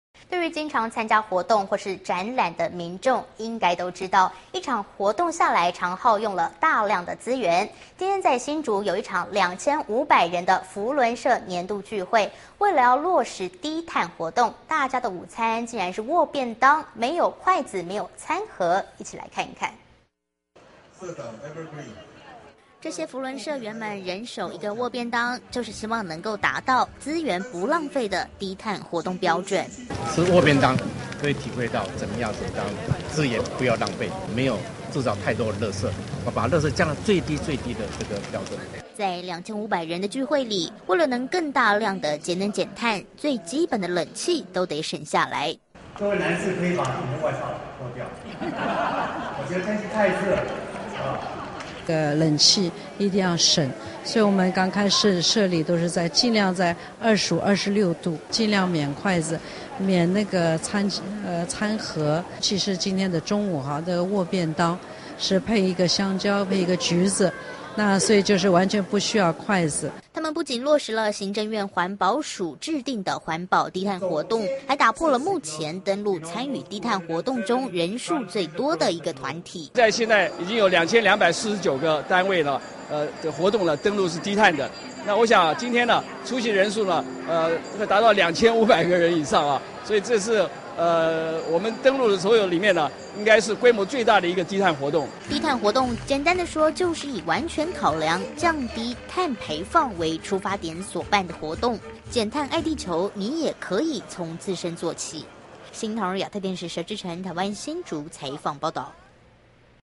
響應低碳活動 扶輪社聚會有創意
【新唐人亞太台2012年4月15日訊】對於經常參加活動或者展覽的民眾應該都知道，一場活動下來常耗用了大量的資源，今天在新竹有一場2500人的扶輪社年度聚會，為了落實低碳活動，大家午餐竟然是握便當，沒有餐盒沒有筷子，一起來看看。

這些扶輪社員們人手一個握便當，就是希望能達到資源不浪費的低碳活動標準。

國際扶輪3500地區總監張宏明：
「吃握便當可以體會到怎麼樣子讓資源不要浪費，沒有製造太多的垃圾，把垃圾降到最低最低的標準。」

在2500人的聚會裡，為了能更大量的節能減碳，最基本的冷氣都得省下來。

「各位男生你可以把你的外套脫掉了，我覺得天氣太熱了。(笑)」

3500區年會籌備主委 苑芝珊：
「冷氣一定要省，所以我們剛開始設立都是在盡量在25 26度，盡量免筷子免那個餐盒，其實今天的中午的握便當是配一個香蕉跟一個橘子，那所以就是完全不需要筷子。」

他們不僅落實了行政院環境保護署制定的環保低碳活動，還打破了目前登錄參與低碳活動中人數最多的一個團體。

行政院環境保護署長 沈世宏：
「在現在已經有2249個單位的活動是低碳的，今天出席人數達到2500個人以上，這是我們登錄的所有的裡面，應該是規模最大的一個低碳活動。」

低碳活動，簡單的說就是以完全考量降低碳排放為出發點所辦的活動。減碳愛地球，你也可從自身做起。

新唐人亞太電視 佘志誠 台灣新竹採訪報導

其他推薦:
《神韻》《SHEN YUN》